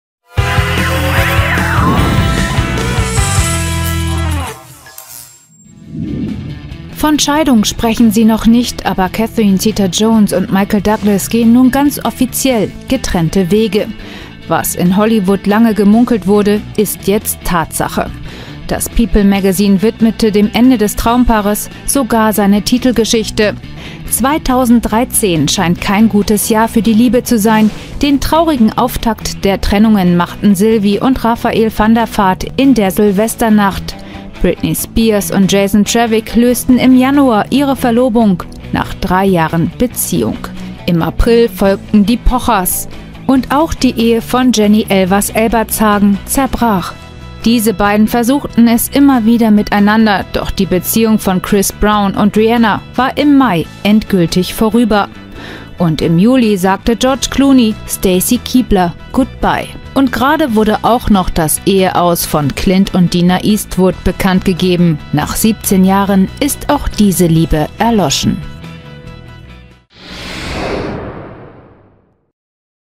2013: kein Jahr der Liebe
Die Beziehungen der Promis gehen reihenweise in die BrÃ¼che.
2013: kein Jahr der Liebe
2013: kein Jahr der Liebe
2013: kein Jahr der Liebe